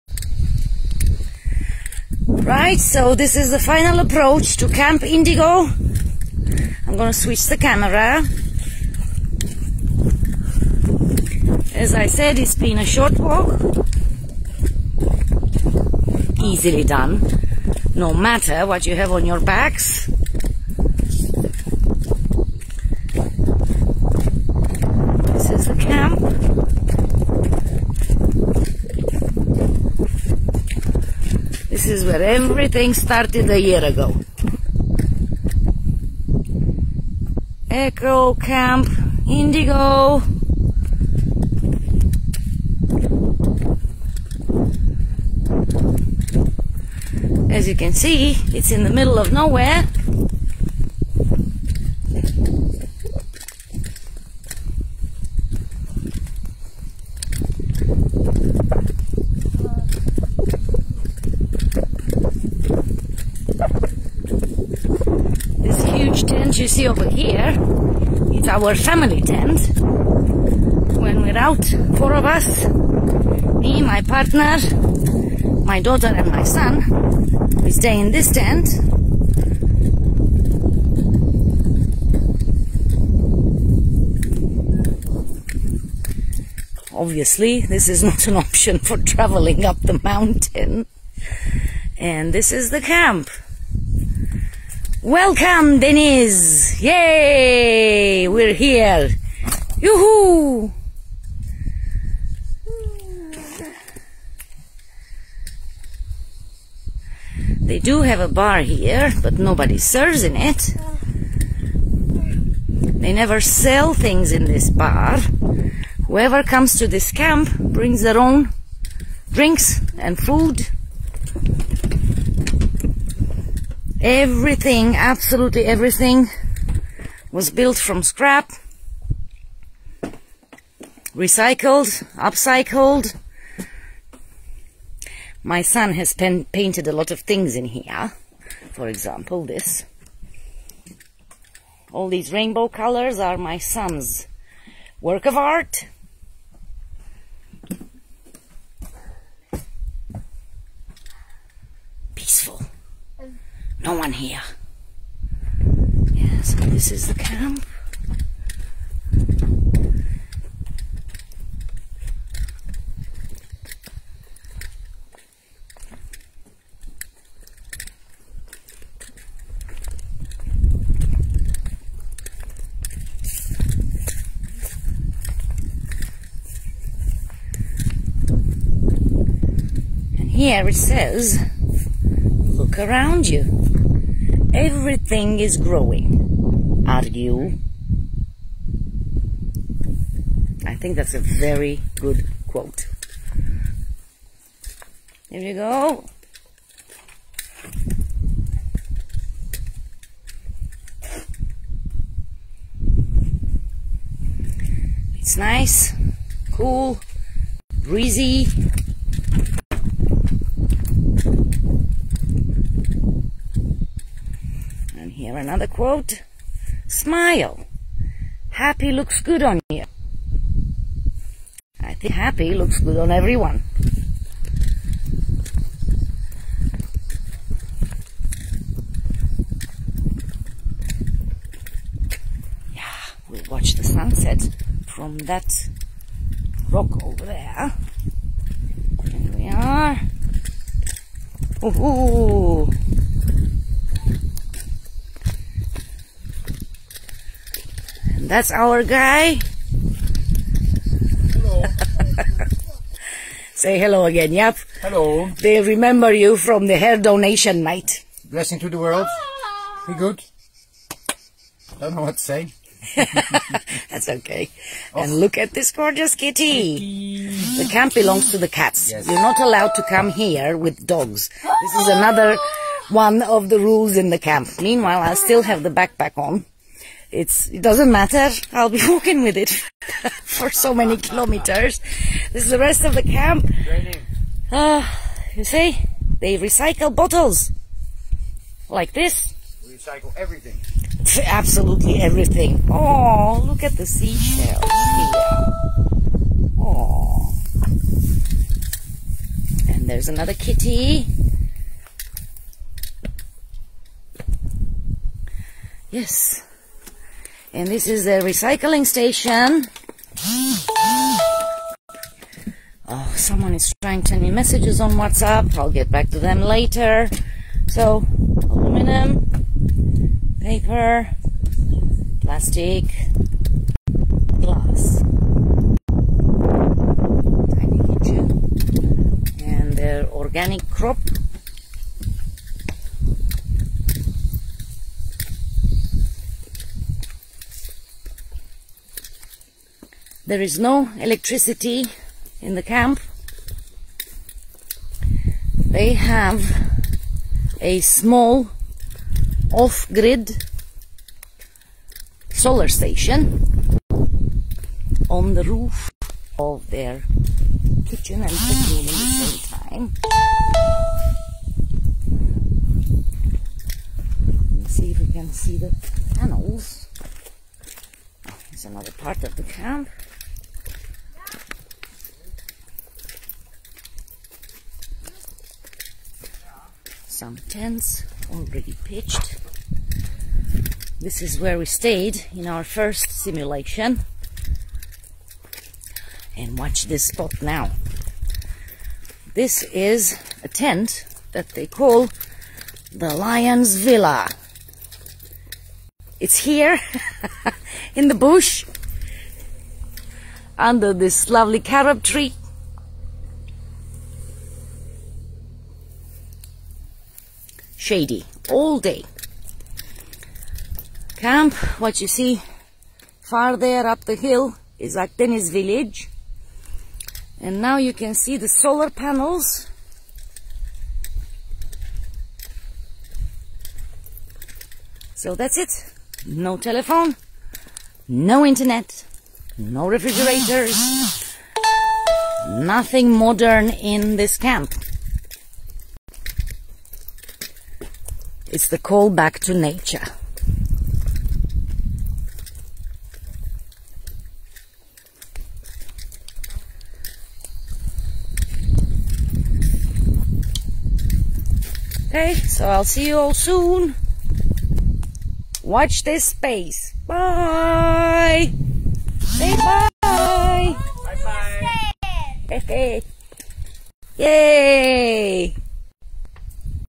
Eco Camp Indigo
14th of July 2021. Not a very good quality video but it will explain a lot. One of our favourite camping spots.